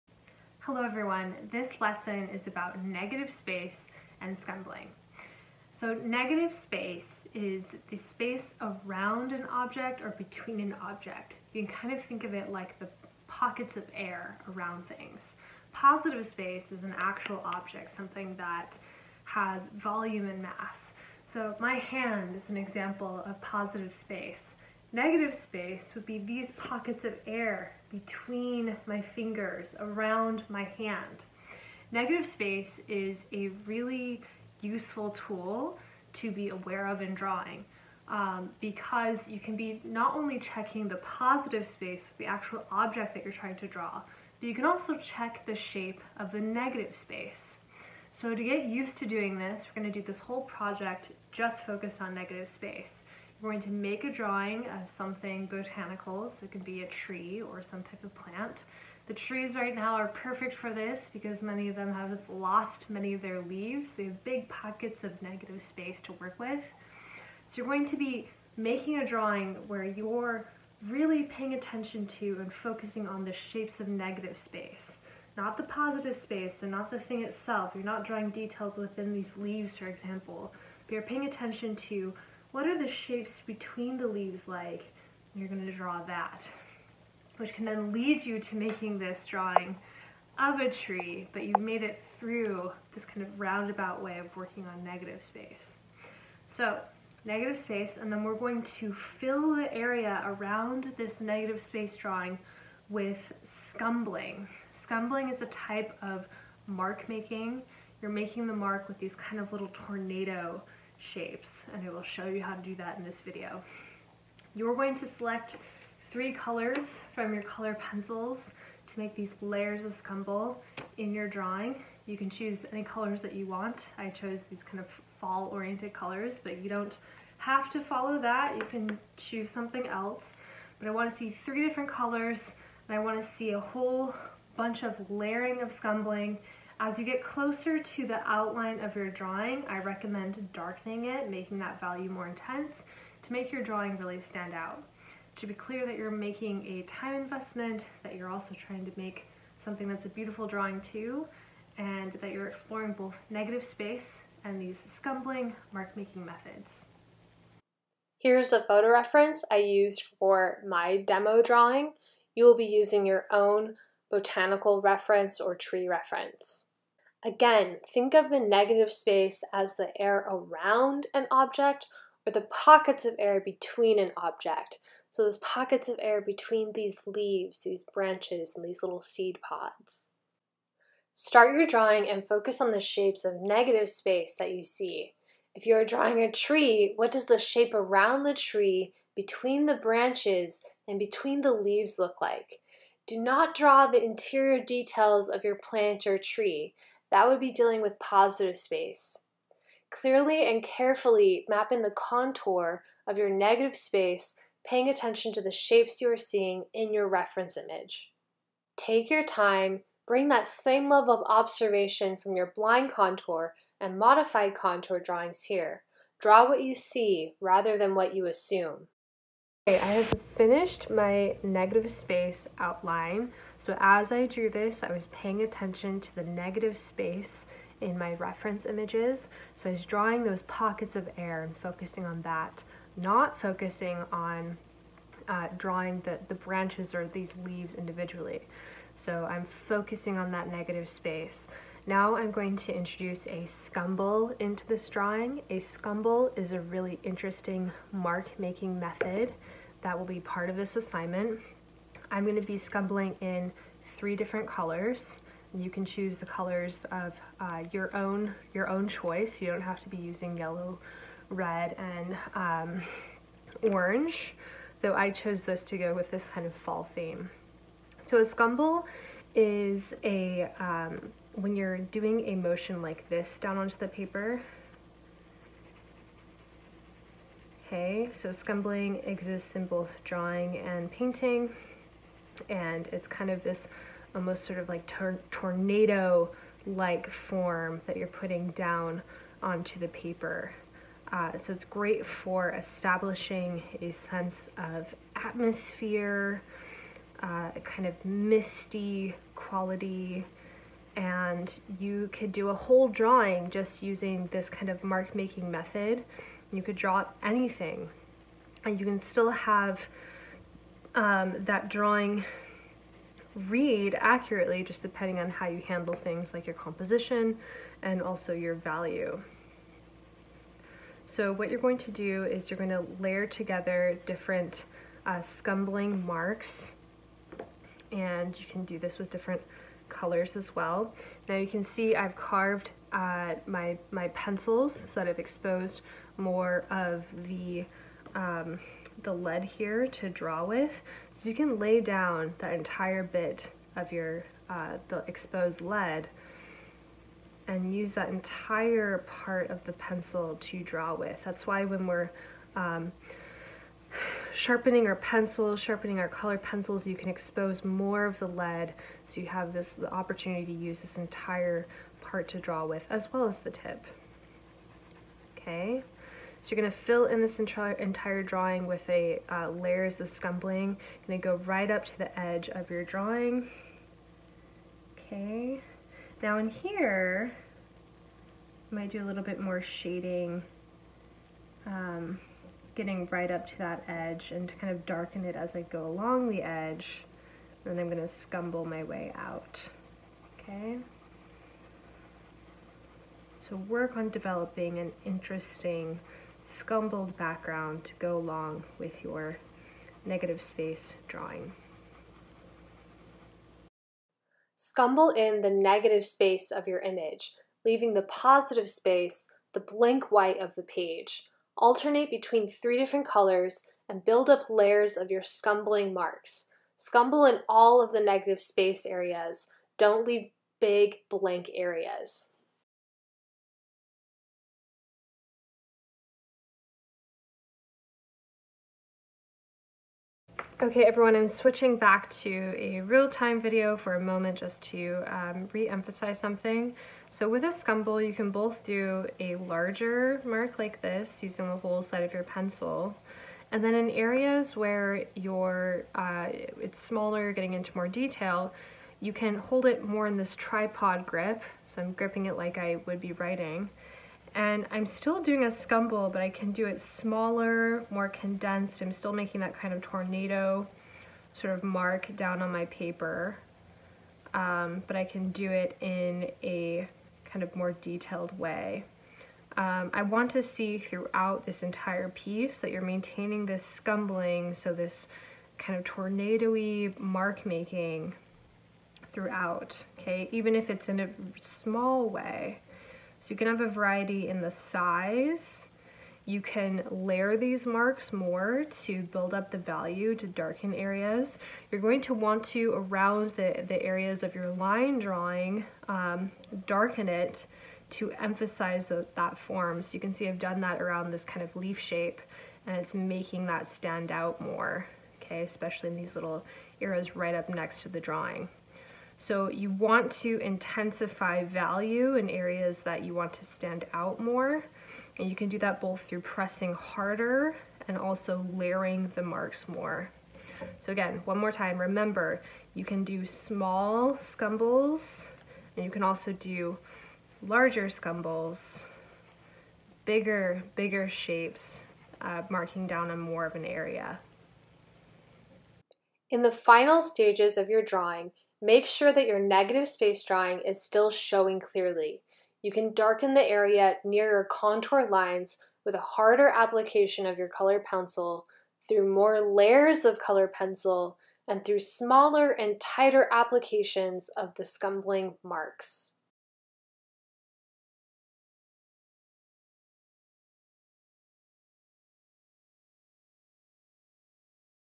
Negative Space Scumble Drawing
In this lesson, we will be learning about positive space versus negative space and an intriguing mark-making method called scumbling.